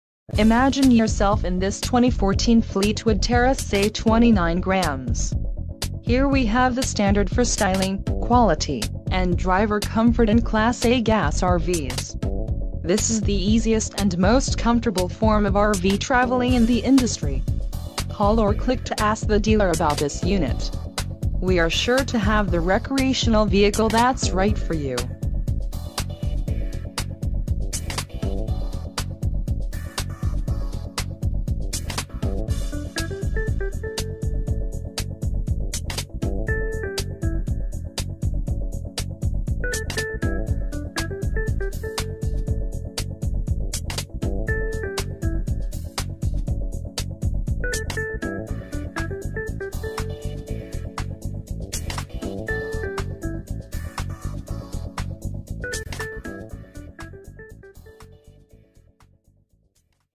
2014 Fleetwood Terra SE Motorhome For Sale near Los Angeles, CA
2014 Fleetwood Terra SE For Sale near Los Angeles, California

Niels Motor Homes of North Hills, CA has this brand new 2014 Terra Se 29G and it\'s itching to hit the road! Equipped with a television in the bedroom and an exterior entertainment center you won\'t miss a second of the game regardless of where you are. Stop by Niels Motor Homes of North Hills to see this Class A Gas RV from Fleetwood first hand! , Electric Awning - Black - (n/a On 31c)\r\nBedroom Lcd Hd Led 19\"\r\nExterior Entertainment Center\r\nSide View Camera Package\r\nGlass Shower Door\r\nSatellite Radio\r\nLuminous Night - Full Body Paint\r\nDual Glazed Windows\r\nHydraulic Levelers